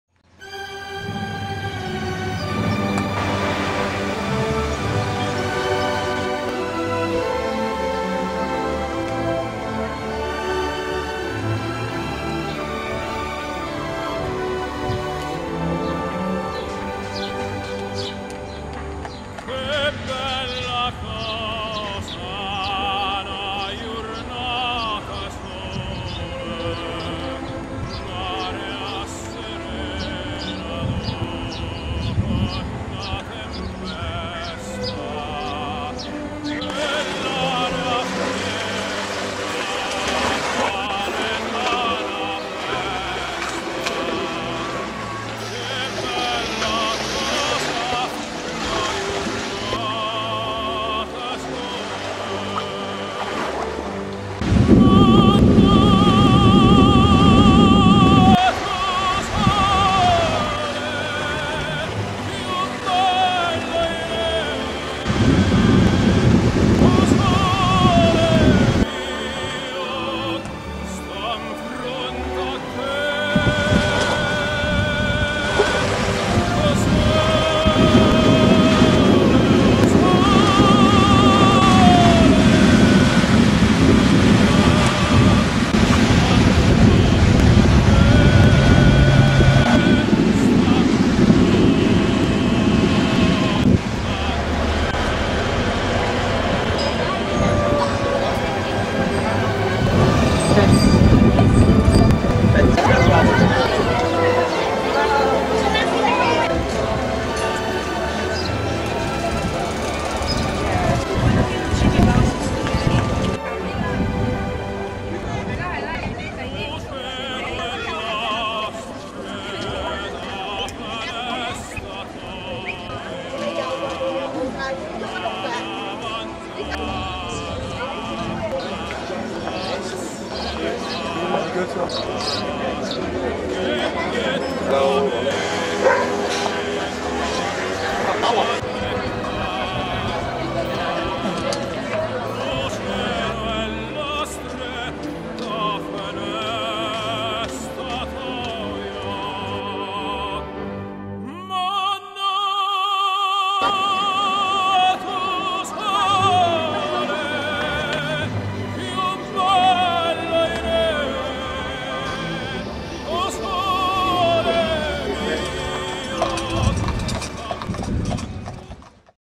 2006 Pisa
2006 Op doorreis naar Rome een tussenstop gemaakt op camping Mare e Sole in Livorno, en een bezoek gebracht aan de scheve toren van Pisa. Een paar dagen later door gereden naar Rome